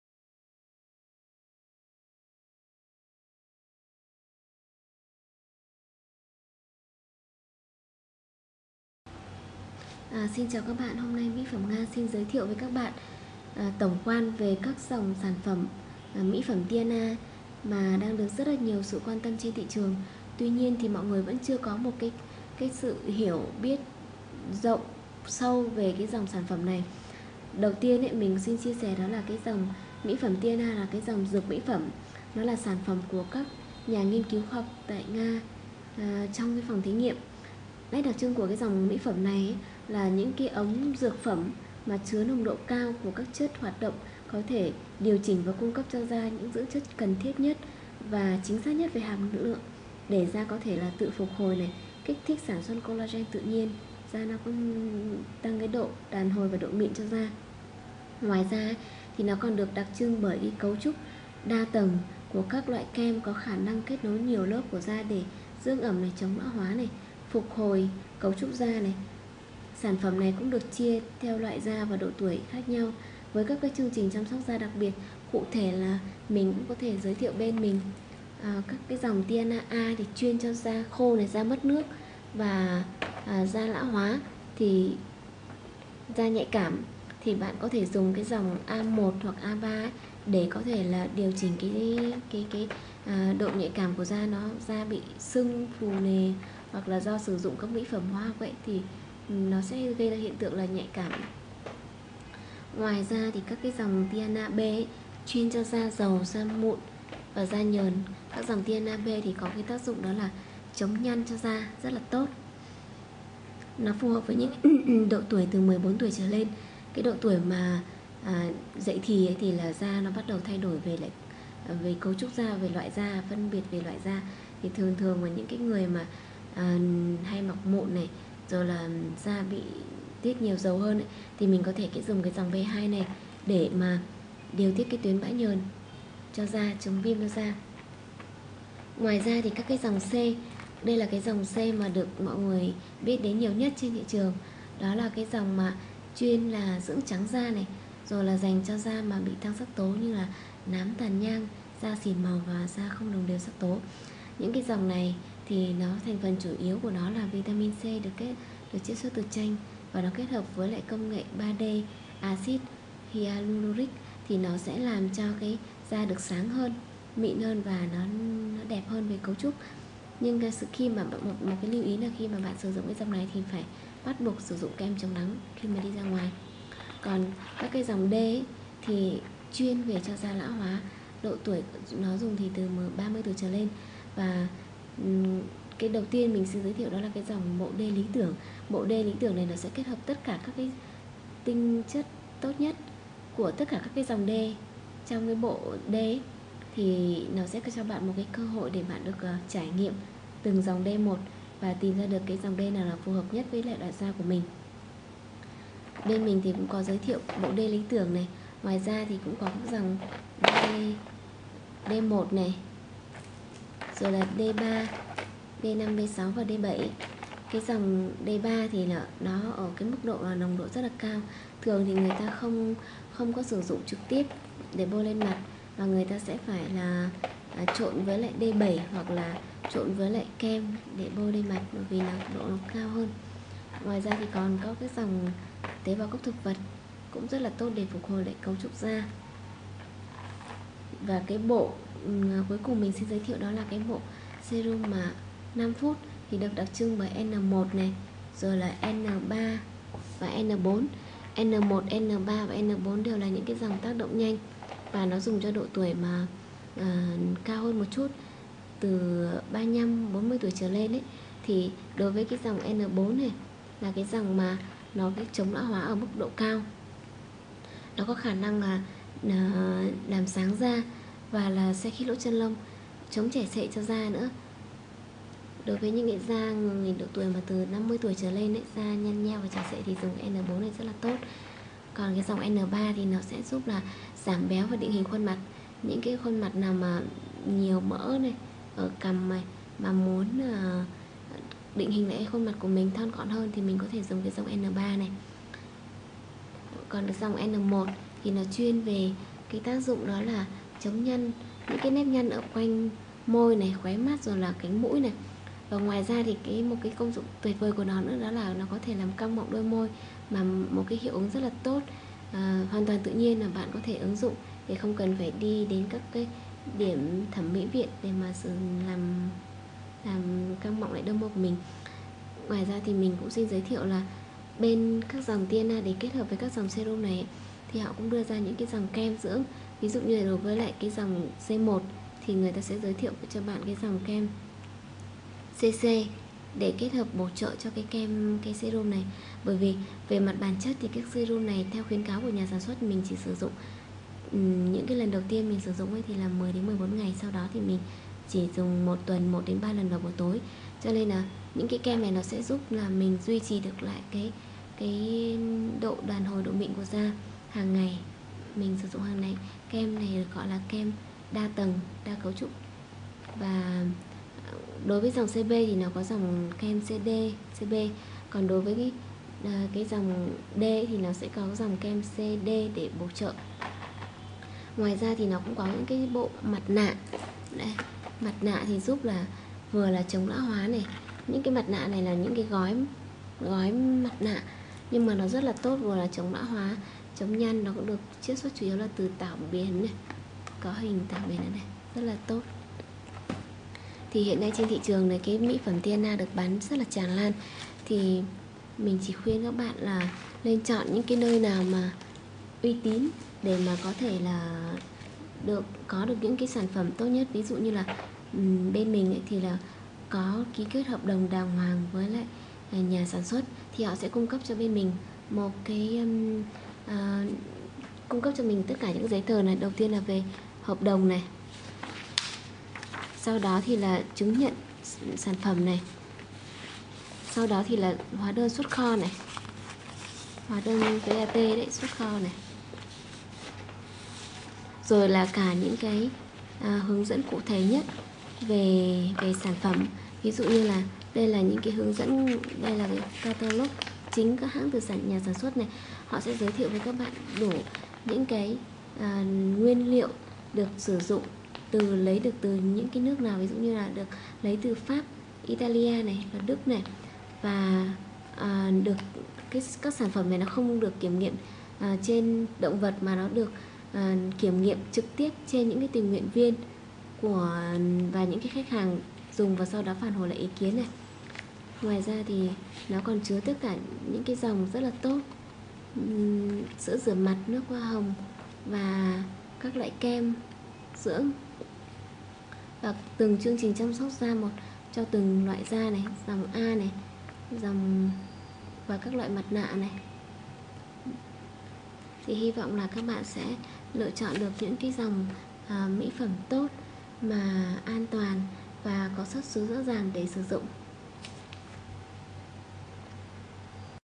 Review về các dòng serum dưỡng & điều trị các vấn đề da của dược mỹ phẩm Teana
Mỹ phẩm Teana là sản phẩm nghiên cứu của các nhà  khoa học dược mỹ phẩm trong phòng thí nghiệm của viện nghiên cứu Liên bang Nga.Sản phẩm là một liệu pháp giúp chăm sóc da tại nhà thay thế cho các chi phí tốn kém tại Spa.Nguyên liệu chính từ tảo biển tươi Đại Tây Dương và được mua thêm từ các nước đi đầu trong công nghệ mỹ phẩm như : Pháp, Y , Đức
Nét đặc trưng của dòng mỹ phẩm này là sản phẩm được đóng dưới dạng ống dược phẩm chứa nồng độ cao của các chất hoạt động có thể điều chỉnh và cung cấp cho da dưỡng chất cần thiết nhất với hàm lượng chính xác nhất để da có thể tự phục hồi và kích thích việc sản sinh collagen tự nhiên cho da.Ngoài ra nó còn được đặc trưng bởi các loại Emulsion đa tầng thuộc các dòng mỹ phẩm thiên nhiên cao cấp giúp sự liên kết và trao đổi chất giữa các lớp của da.
Sản phẩm của mỹ phẩm Teana rất đa dạng nó chia theo loại da và độ tuổi khác nhau và đặc biệt nó đưa ra các chương trình chăm sóc da phù hợp với lứa tuổi và vấn đề da khác nhau :

Các dòng "Serum teana tươi A " -chuyên chăm sóc da khô,da mất nước, da bị dị ứng do mỹ phẩm hóa học , da nhạy cảm hay bị sưng khi sử dụng mỹ phẩm
Các dòng "Serum Teana tươi B " chuyên chăm sóc da nhờn , da mụn , da bị viêm nhiễm, giúp điều tiết tuyến bã nhờn và chống viêm ,ngăn ngừa mụn cho da , se khít lỗ chân lông , thích hợp với độ tuổi từ 14 trở nên
Các dòng "Serum Teana tươi C1 " dưỡng trắng da và dành cho da không đồng đều sắc tố như nám và tàn nhang, sỉn màu
Các dòng "Serum Tean tươi D " : Chuyên chống lão hóa cho da từ độ tuổi 30 giúp chống nhăn và ngăn ngừa lão hóa
Các dòng "Serum 5 phút " chuyên về chống lão hóa da ở cấp độ cao , định hình và thon gọn khuôn mặt , căng mọng môi và giảm nhăn ở khóe mắt , cánh mũi và quanh miệng.
Các dòng  "Serum TEANA TƯƠI IPF "  Nâng cơ , chống nhăn và lão hóa da

Ngoài các dòng serum collagen tuoi ,Teana còn có các sản phẩm kem dưỡng đi cùng với từng dòng để bổ trợ cho các serum như kem dưỡng trắng da TEANA CC , Kem dưỡng trị mụn Teana CB, Kem dưỡng chống lão hóa Teana CD.
Các sản phẩm của Teana đều được kiểm nghiệm trực tiếp trên các tình nguyện viên chứ không phải trên động vật nên độ an toàn cao hơn và hiệu quả rõ rệt hơn.